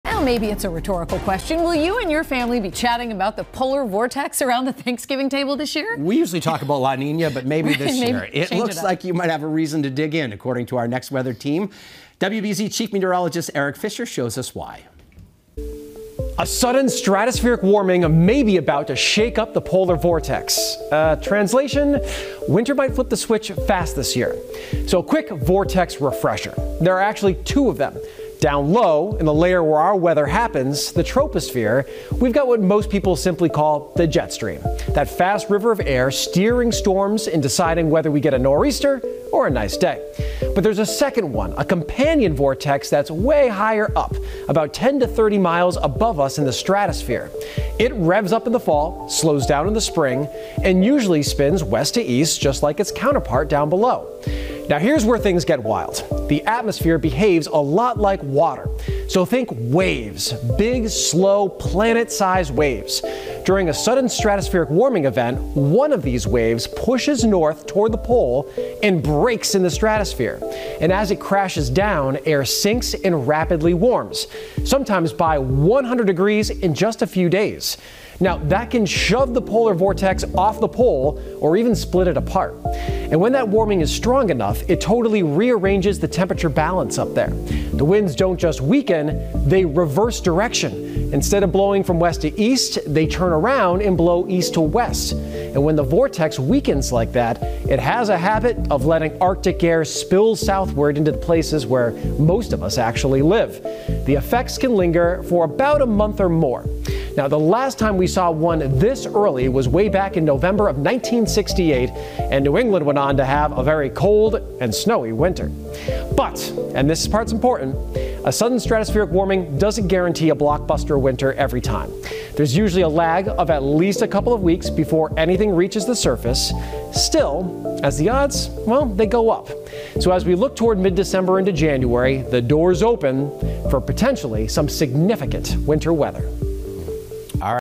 What is the polar vortex? The science behind the sudden shift in temperature
WBZ-TV chief meteorologist Eric Fisher explains why the door is opening for some potentially significant winter weather.